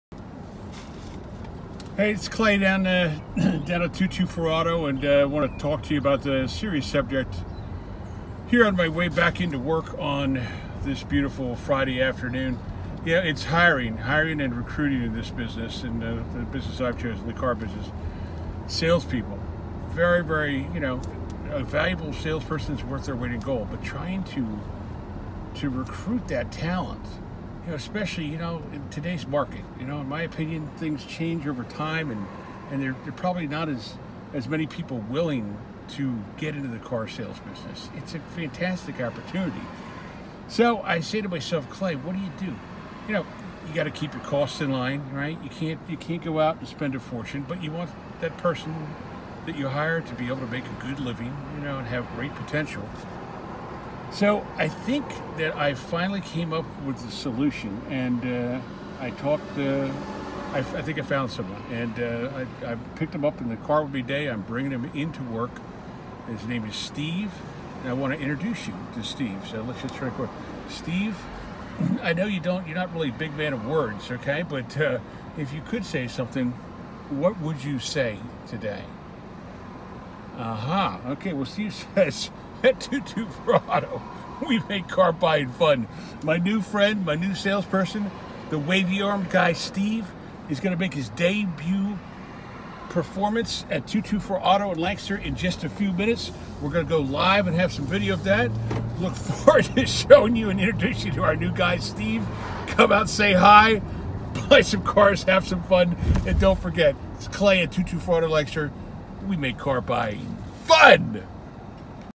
Meet “Steve” The wavy armed guy our newest salesperson!
Recruiting and hiring in the car business is not easy, but I think I’ve found the answer. Please join me in welcoming our newest team member at 224Auto in Lancaster and Ephrata... Steve!
Steve works for peanuts and is full of more hot air than your average Salesperson... sometimes. 🤷🏼‍♂️
Come find out why your friends and neighbors are already 224Auto4Life!
Anthony Silver Anthony Joseph Scheuerman Clayton Blevins Howard Smith William Blevins Landin Jared Homan Don Reider Arky Nana